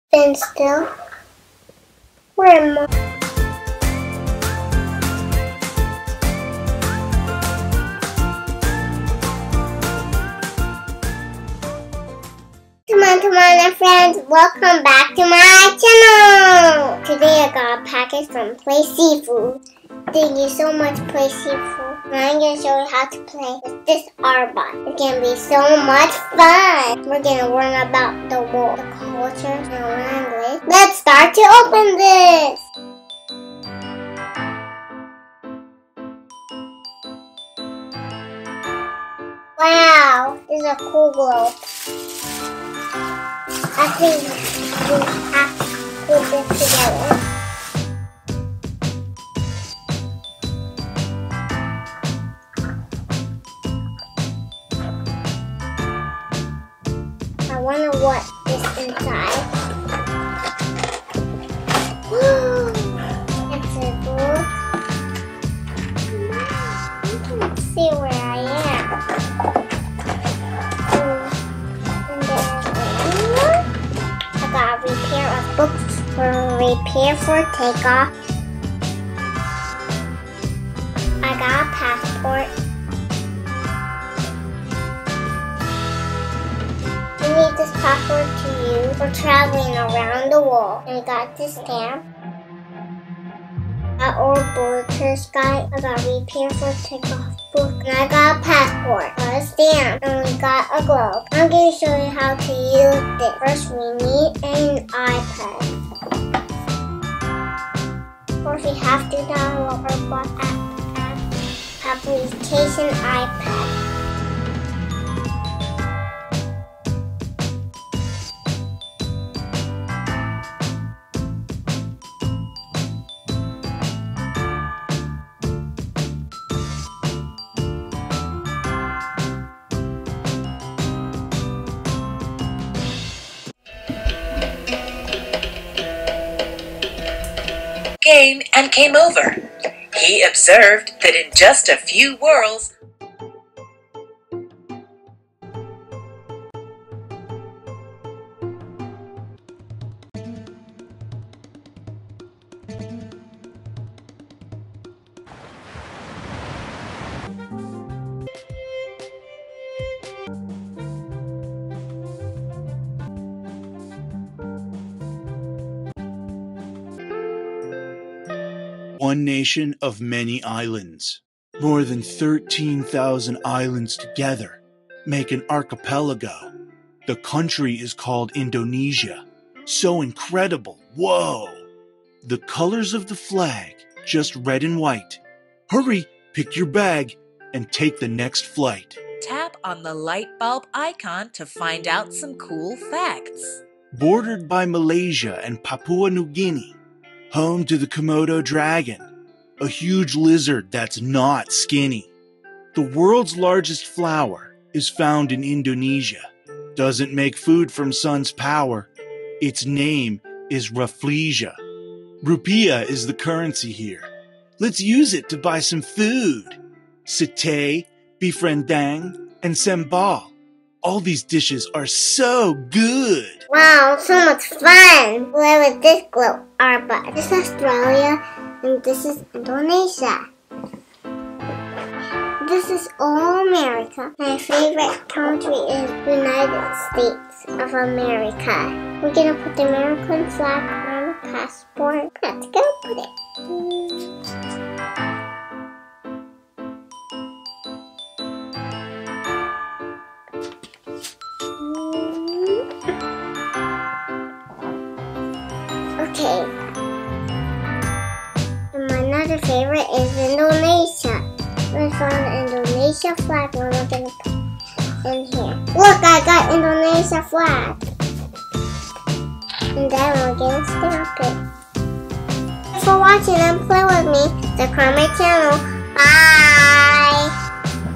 UNBOXING SHIFU ORBOOT | STEM TOYS | EDUCATIONAL TOYS
Hello Friends today we're unboxing the STEM Toys from Playshifu this is so much fun to
Travel the world with Shifu Orboot
The educational, Augmented Reality globe always amazes the curious little minds. Fun and interactive, this adventurous ride helps your child to absorb endless knowledge at the tap of a finger.